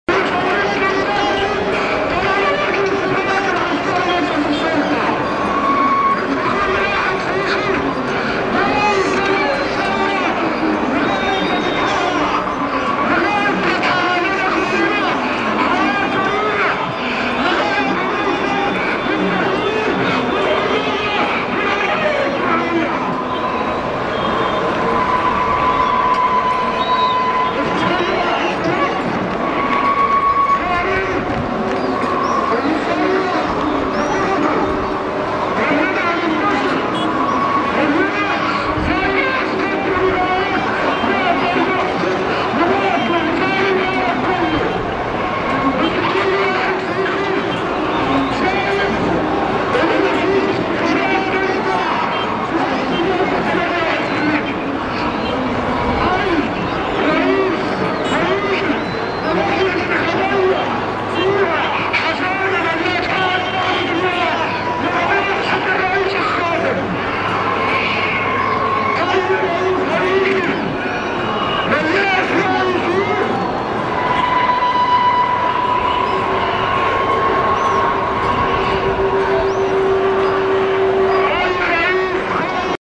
Tahrir overview 19.00
There is yet the march that is rumored to be the biggest which is at least quarter of an hour away